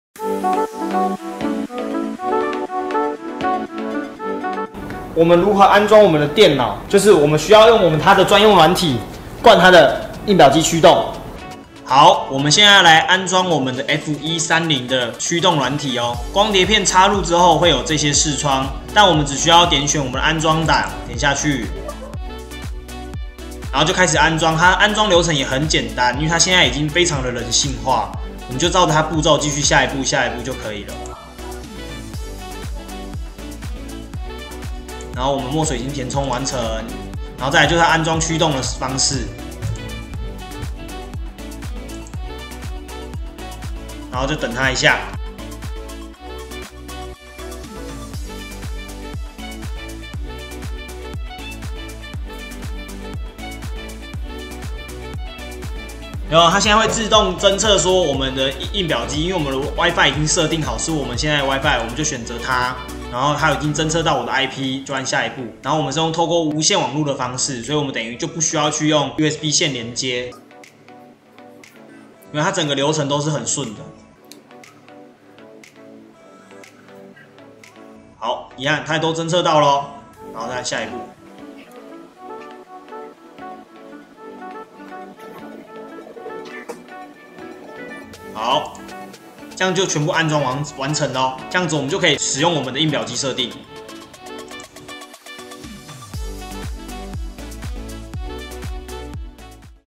EPSON SC-F130 桌上型熱昇華印表機驅動安裝教學(純教學)|熱昇華印表機推薦|熱轉印設備推薦 | 奕昇有限公司
✨EPSON SC-F130熱昇華印表機✨
驅動安裝教學🪐

透過EPSON軟體光碟安裝驅動程式超簡單👇👇👇
1⃣安裝Install
2⃣同意合約條款
3⃣傳送使用資訊至Epson
4⃣已將墨水填充至大容量墨水槽
5⃣透過無線網路(Wi-Fi)連結
6⃣選擇Wi-Fi位址
7⃣完成

軟體安裝完成後
EPSON SC-F130可直接透過Wi-Fi與裝置連接
搭配熱昇華多功能機輕鬆轉印多種商品✌

歡迎聯繫洽詢EPSON SC-F130☎
讓轉印更快更便利🧡